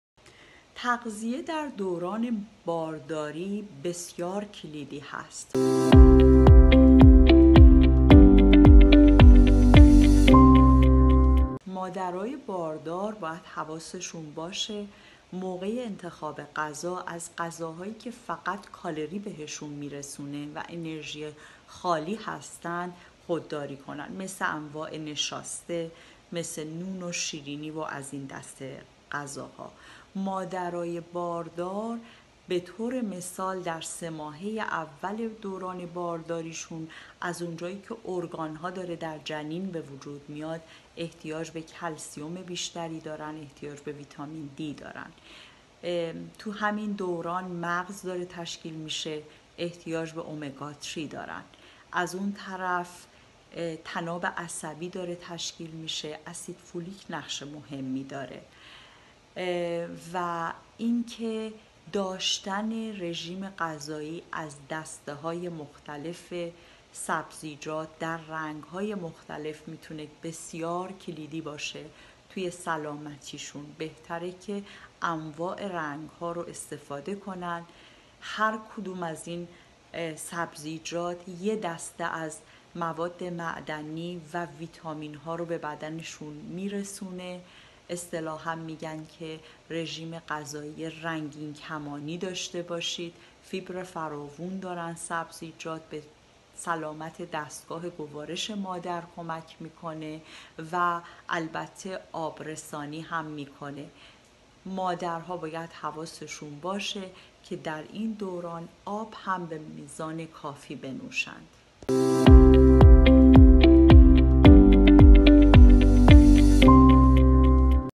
تغذیه در دوران بارداری
تغذیه در دوران بارداری نقش بسزایی در سلامت مادر و نوزاد دارد.